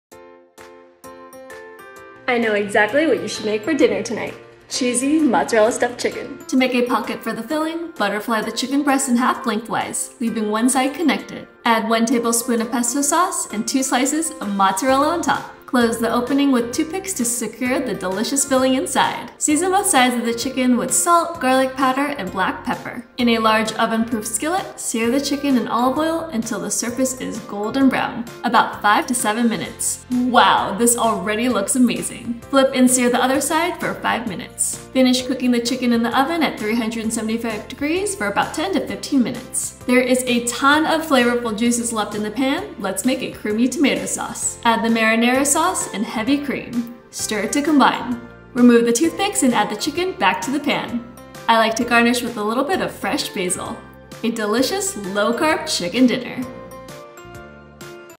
Cheesy Mozzarella Stuffed Chicken Breast Recipe 🍗
This mouthwatering recipe showcases tender chicken breasts stuffed with creamy mozzarella for a cheesy and flavorful dish.

📕 MY COOKBOOK

• Cutting Board
• Vitamix Blender
• Knife Set
• Glass Bowls
• Food Processor
• Kitchen Scale